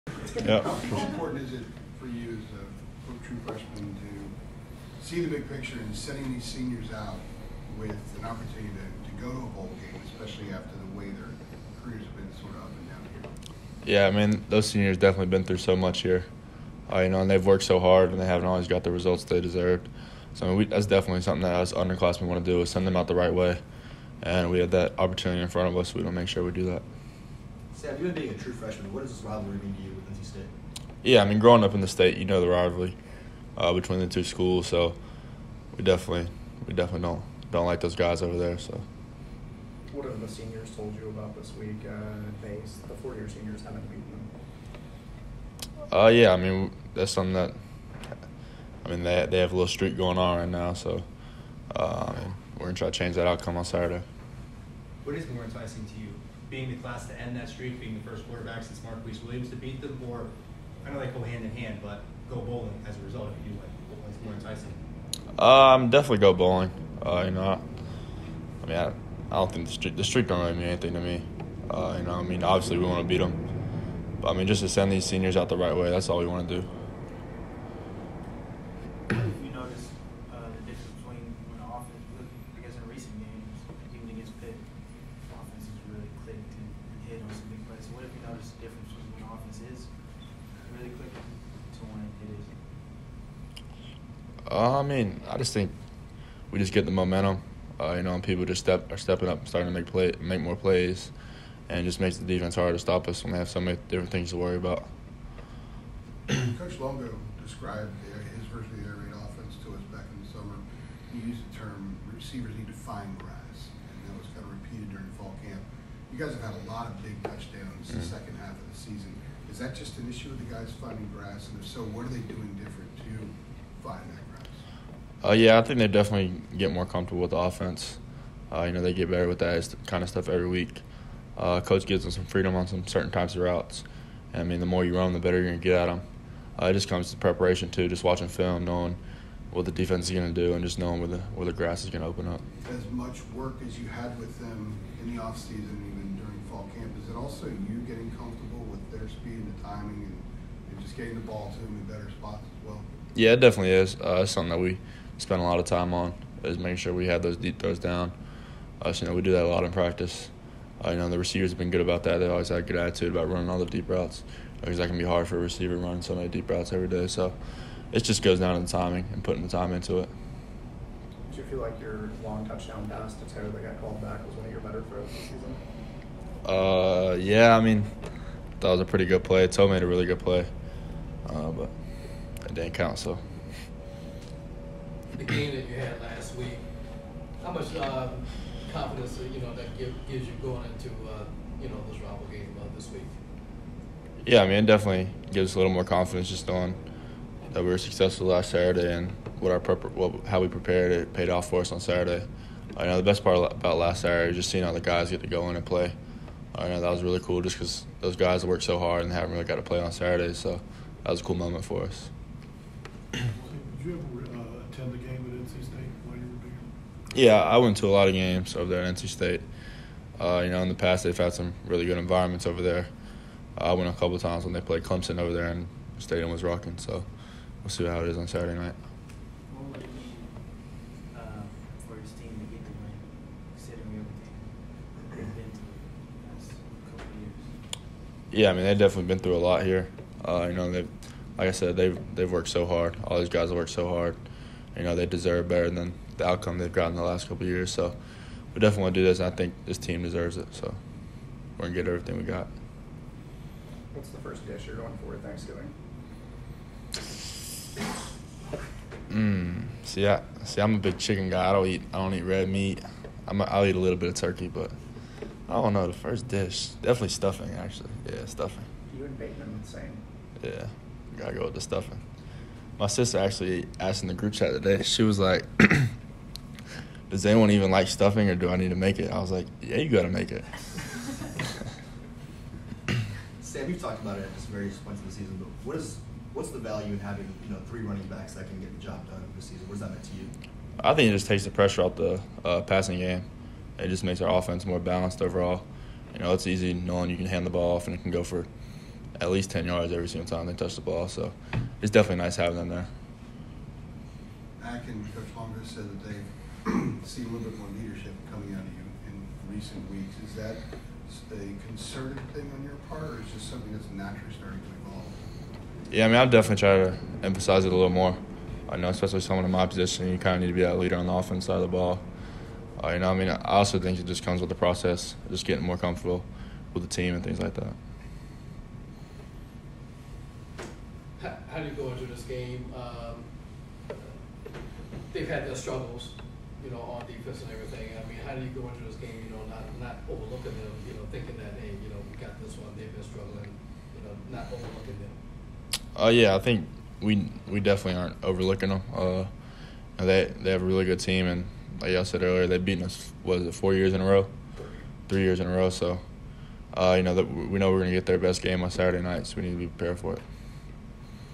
ICTV: Sam Howell Press Conference Pre-NC State
The UNC quarterback spoke to reporters Tuesday afternoon ahead of the NC State game.